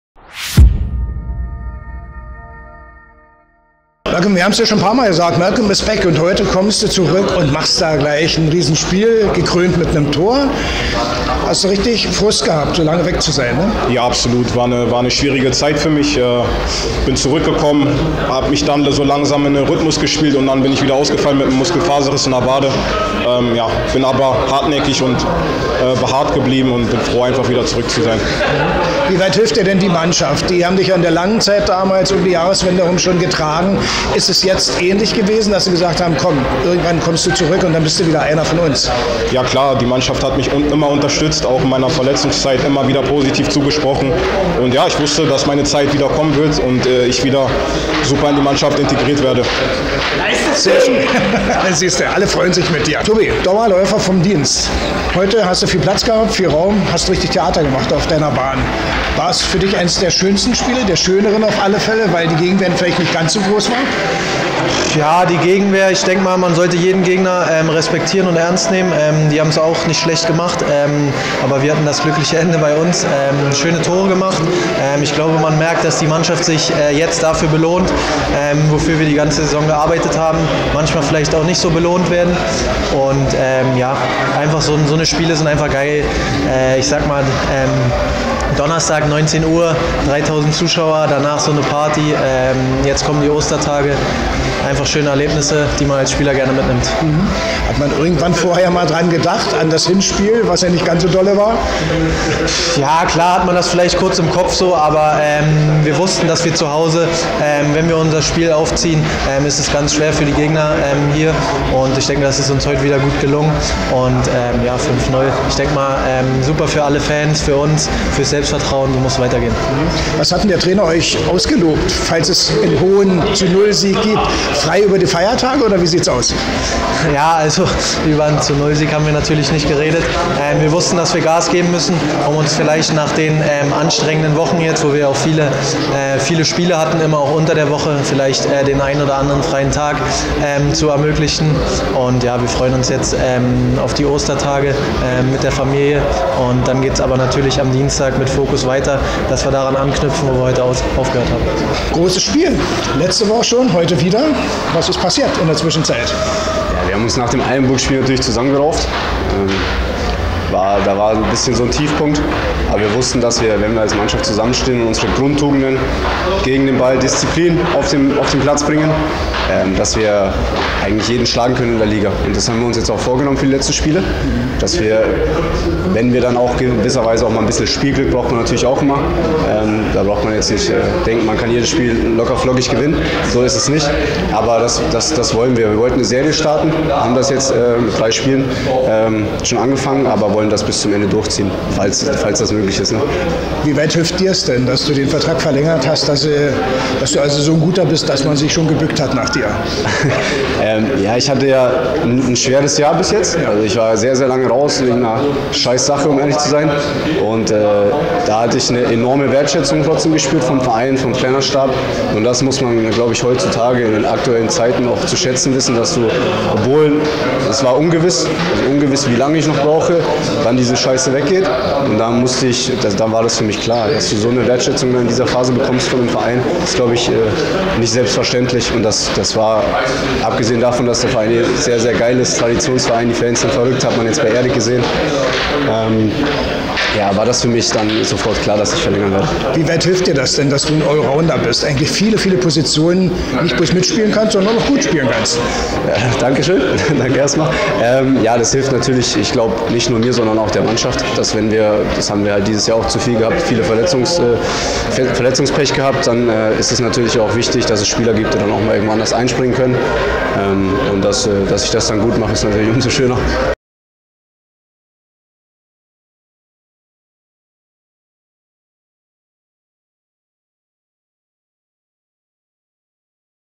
FC Energie Cottbus | Torschütze Malcolm Badu, Dauerrenner Tobias Hasse, Vorlagengeber Jonas Hofmann
Der FC Energie Cottbus siegt 5:0 gegen Tasmania Berlin, Malcolm Badu kommt zu einem Jokereinsatz und macht ein Tor, Tobias Hasse ist der Dauerrenner beim FCE und Jonas Hofmann glänzte schon in Halberstadt und dieses Mal wieder als Vorlagengeber. Alle drei bei uns  im Interview nach dem Spiel.